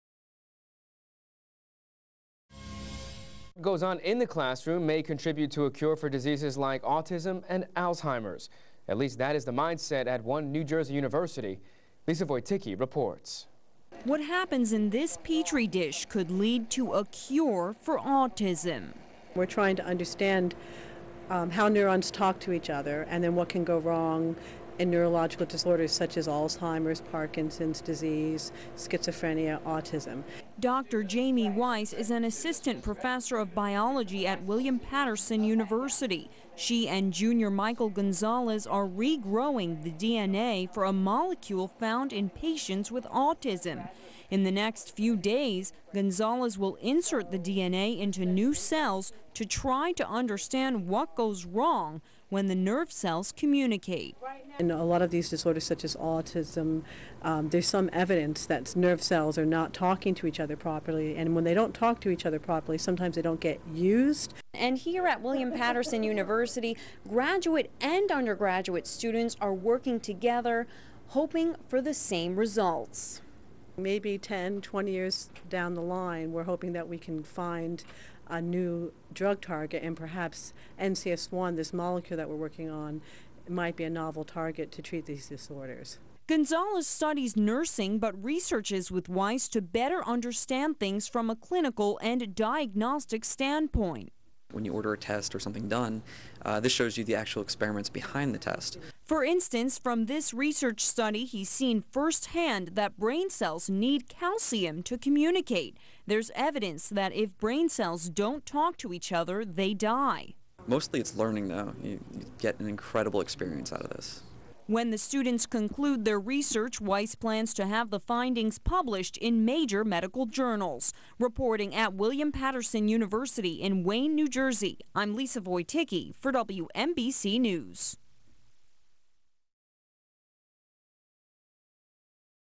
William Paterson University Biology Professor and Nursing Student Work Side-by-Side
Jamie Weiss, Ph.D., William Paterson University assistant professor of biology, and undergraduate nursing student Michael Gonzalez are featured on news segment with reporter Lisa Voyticki. Weiss primarily works with how brains talk to one another, particularly in the areas of learning and memory, and what goes wrong with the brain in neurological disease. Implication of her research can translate to the clinic by providing information to the development of new treatments for diseases such as Alzheimer's, Autism, and schizophrenia. She includes undergraduate and graduate students in her work, which provides unique research opportunities for industrious William Paterson students.